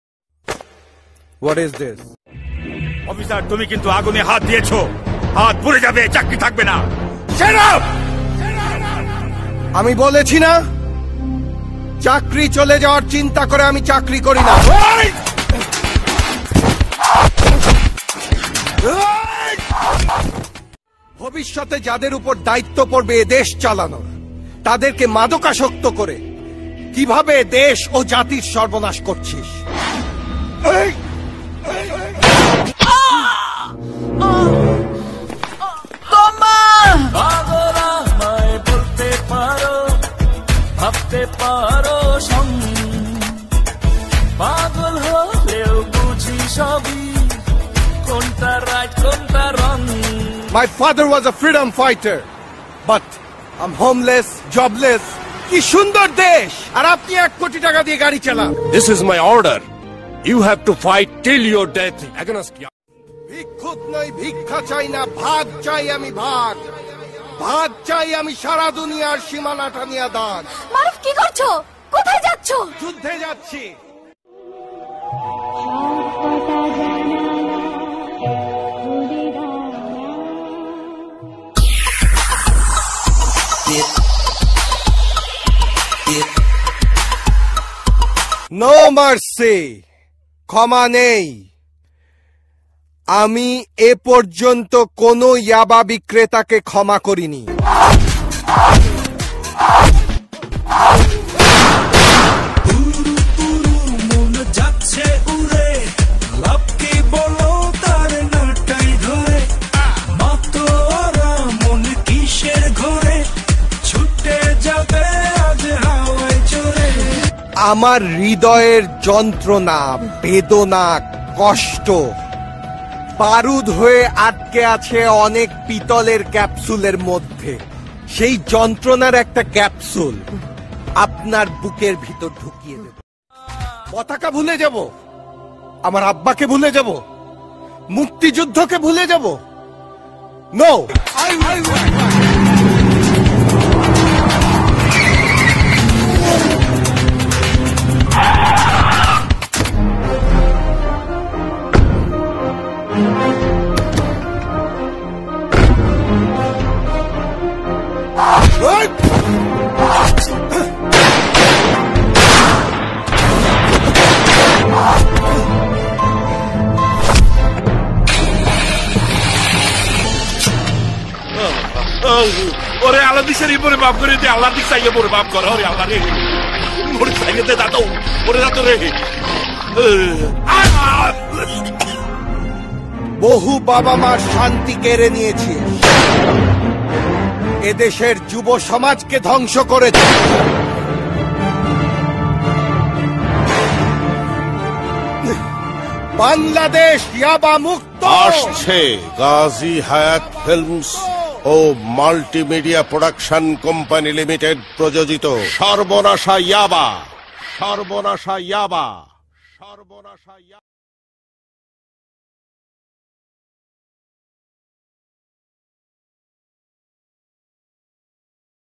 Shorbonasha Yaba Trailer l Shafiul Islam l Naila Nayem l Virat Kholi l Najmul Hasan Papon l Ms dhoni
This video is for fun Bangladeshi fast bowler shafiul Islam is in the main roll of this  video and it's also have Naila Nayem Virat Kholi bcb president Najmul Hasan Papon and Ms dhoni